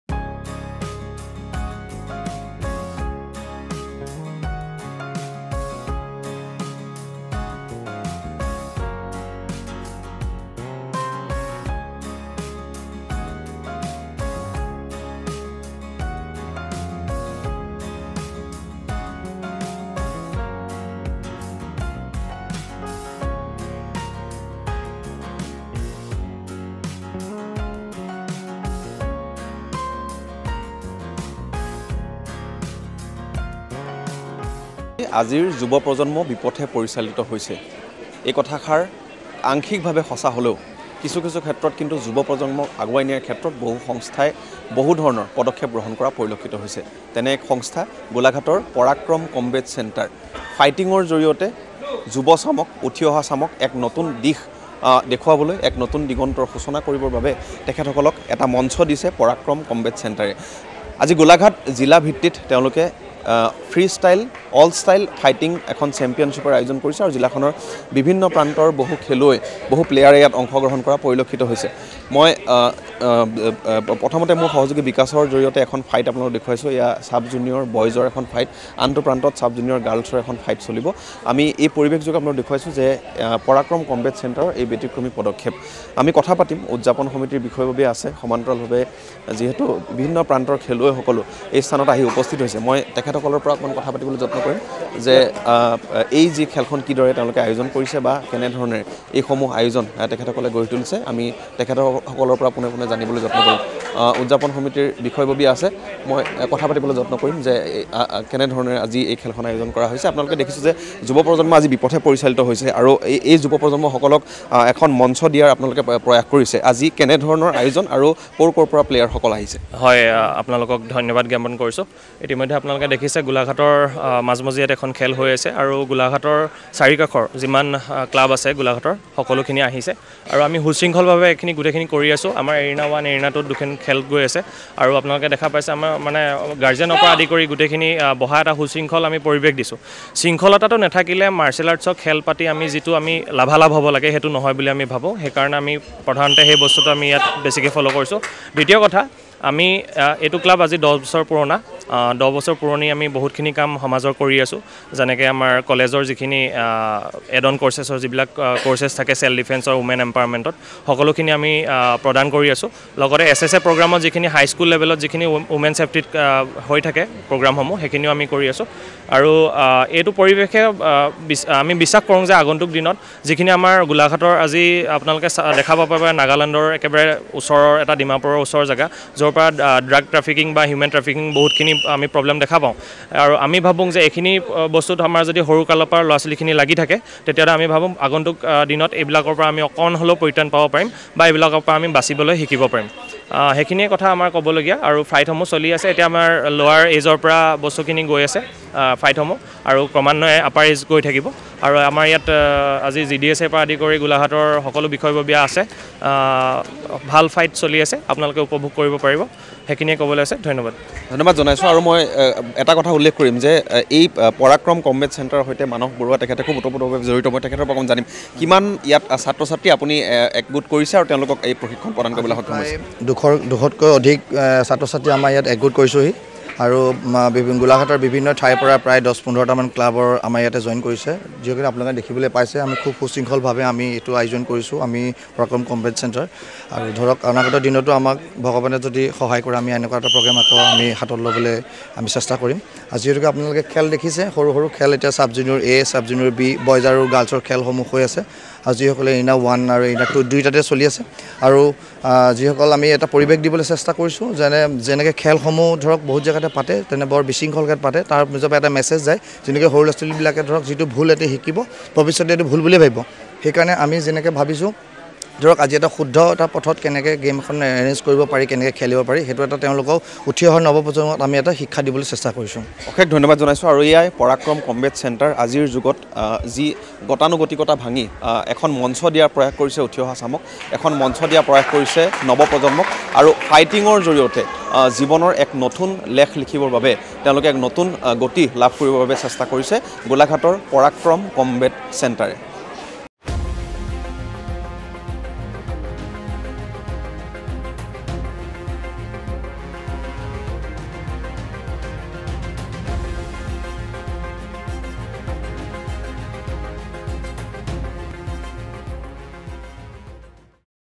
গোলাঘাটত আয়োজিত হৈছে ফ্ৰীষ্টাইল ফাইটিং চেম্পিয়নশ্বিপ
▶️ প্ৰতিযোগিতাখনত জিলাখনৰ বিভিন্ন প্ৰতিযোগীৰ অংশগ্ৰহণ